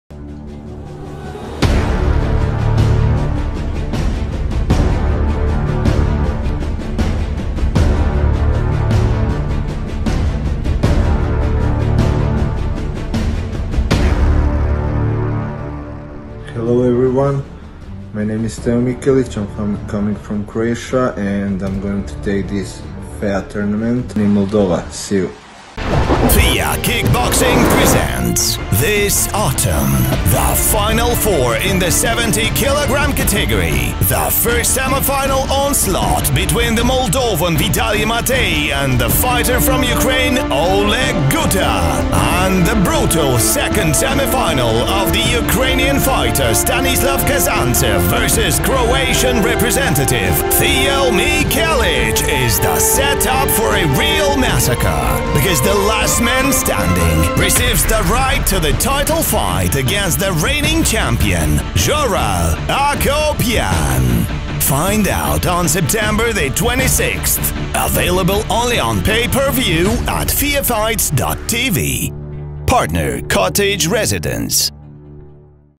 Teo Mikelic - I am from Croatia and I’m going to take part in the FEA tournament. See you!
“Hello everyone. My name is Teo Mikelic. I am from Croatia and I’m going to take part in the FEA tournament in Moldova. See you! "- said Mikelic.

Quick reminder that on September the 26th, within FEA Kickboxing tournament, will take place the 4-man Grand Prix in the lightweight category. Teo Mikelic and Stanislav Kazantsev will meet in the second semifinal. The winner of this bout will challenge the winner of the fight between Matei and Guta on the same evening.

We will find out who will win the trophy on September the 26th. Don't miss it, only at feafights.tv

„Salutare! Numele meu e Teo Mikelic. Sunt din Croația și voi participa la turneul FEA din Moldova. Ne vedem! "- zice Mikelic.

Reamintim că la 26 septembrie, în cadrul turneului FEA Kickboxing, va avea loc piramida Grand Prix în categoria ușoară. Teo Mikelic și Stanislav Kazantsev se vor întâlni în a doua semifinală. Câștigătorul acestui meci îl va provoca pe învingătorul luptei dintre Matei și Guta în aceeași seară.

Vom afla cine va câștiga trofeul pe 26 septembrie. Nu ratați, doar la feafights.tv

"Всем привет! Меня зовут Тео Микелич. Я из Хорватии, очень скоро я буду принимать участие на турнире FEA Kickboxing в Кишиневе. Увидимся " Заявил Микелич.

Напомним, что 26 сентября пройдет турнир FEA Kickboxing в рамках которого состоится Гран-при четверка в легкой весовой категории. Во втором полуфинале встретятся Тео Микелич и Станислав Казанцев. Победитель данной схватки бросит вызов в тот же вечер победителю боя Матей версус Гута.

А кто станет обладателем трофея мы узнаем 26 сентября. Не пропустите, только на feafights.tv